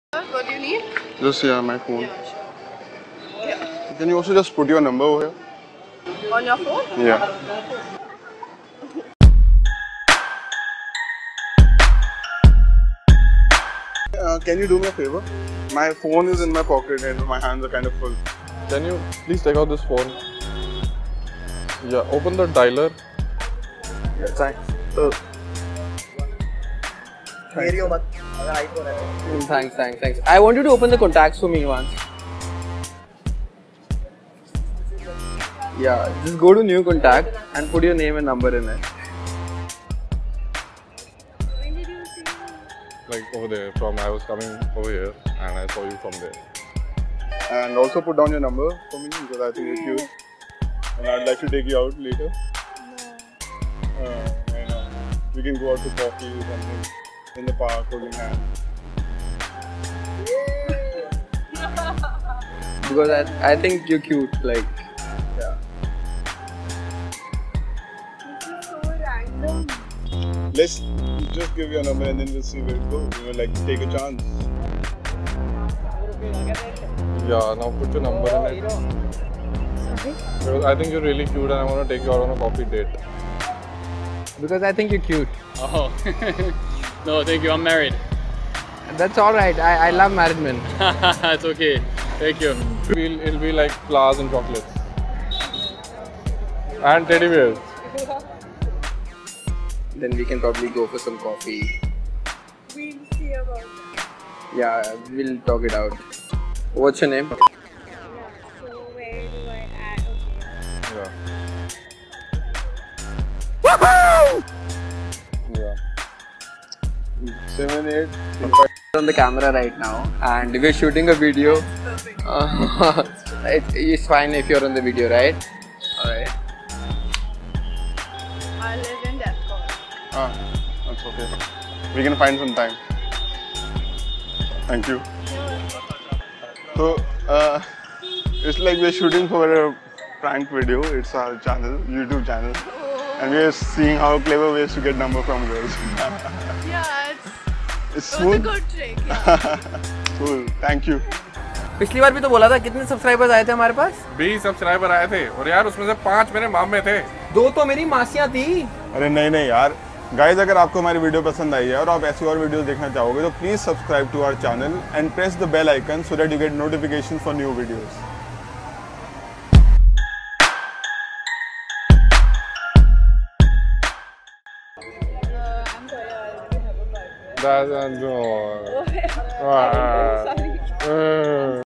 HOW TO GET A GIRL'S NUMBER FAST AND SIMPLE TRICK || All in one
All in one goes on street to find out clever ways on how to get girls' numbers.